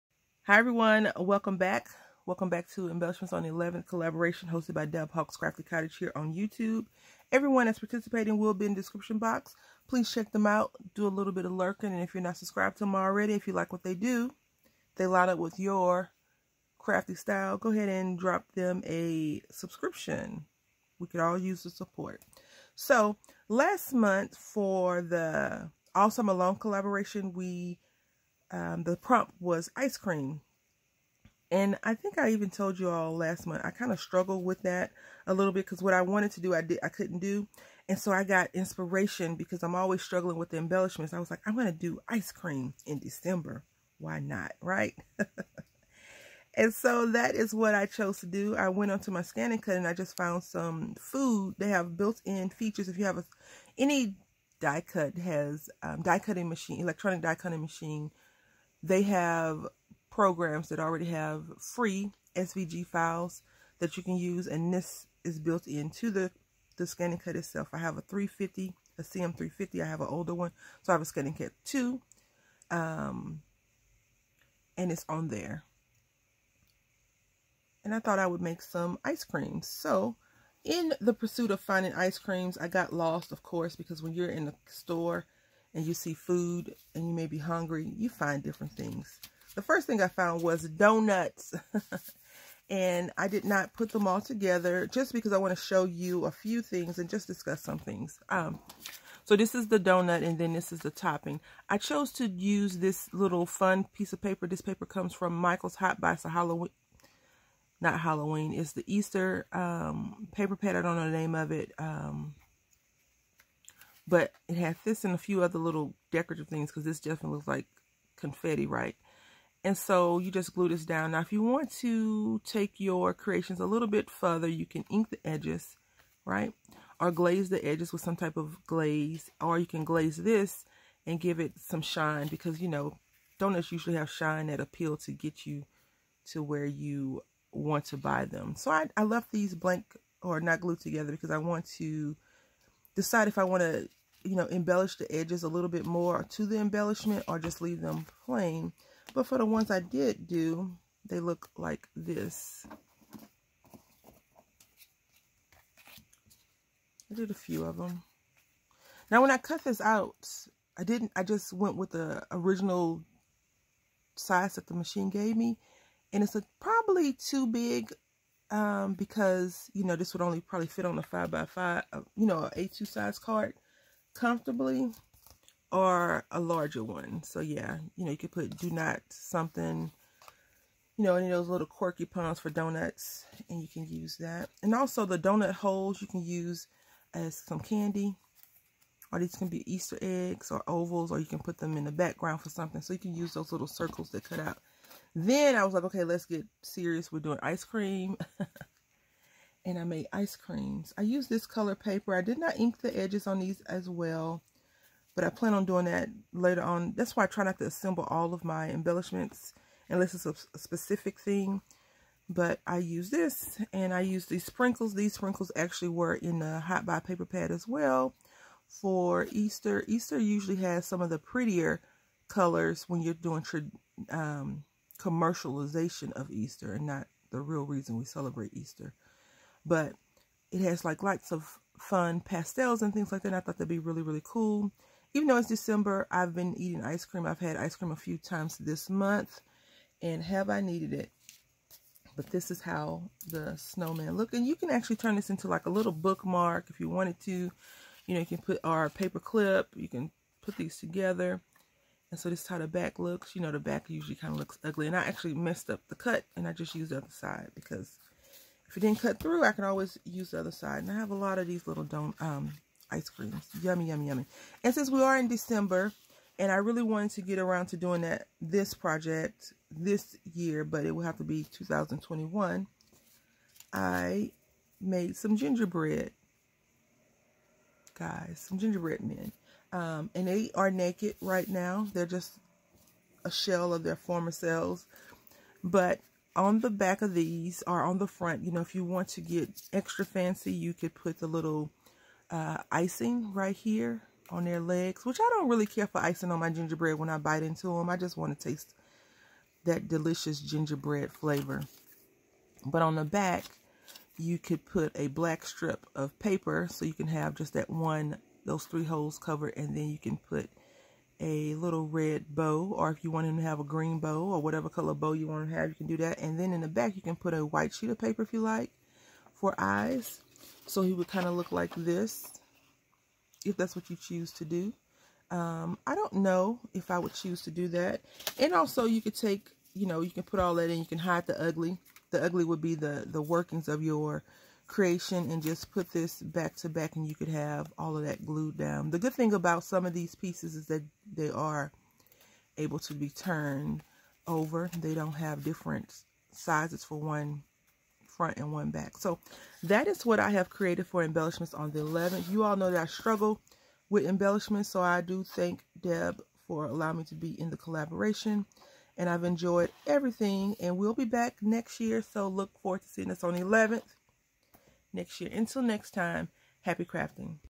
EMBELLISHMENTS ON THE ELEVENTH 2020 (December)
Hi everyone! Welcome back.  This collaboration is will be us creating embellishments on the 11th of the month for the entire year.

Deb Houck's Crafty Cottage               dahliadesigns57
Crafts-I-Heart                                        plan.craft.love
Lynnea Berr                                           lynneaberr
RareRoo Crafts                                     rareroo_crafts